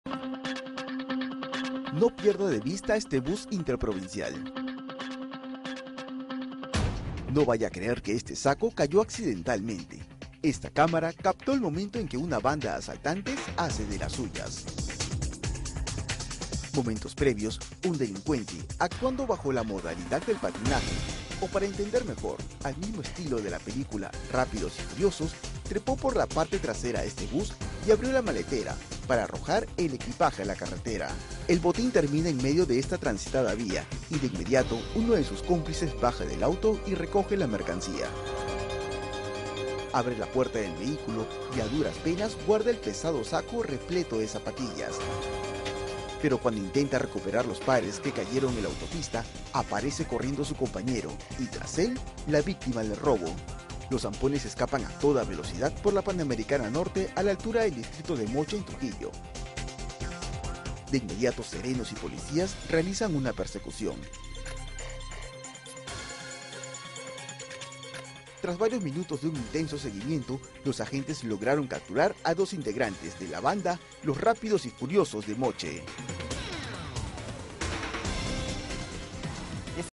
Trujillo: ladrones asaltan bus en movimiento en la Panamericana Norte
Un delincuente, actuando bajo la modalidad del "patinaje", o para entender mejor, al mismo estilo de la película “Rápidos y Furiosos”, trepó en la parte trasera de bus interprovincial y abrió la maletera para arrojar el equipaje a la carretera.

El botín terminó en medio de la transitada vía y de inmediato uno de sus cómplices bajó de un auto y recogió la mercancía. Tras cometer el robo, los hampones escapan a toda velocidad por la Panamericana Norte a la altura del distrito de Moche en Trujillo.

De inmediato serenos y policías realizan una persecución. Tras varios minutos de un intenso seguimiento, los agentes lograron capturar a dos de los integrantes de la banda denominada “Los Rápidos y Furiosos de Moche”.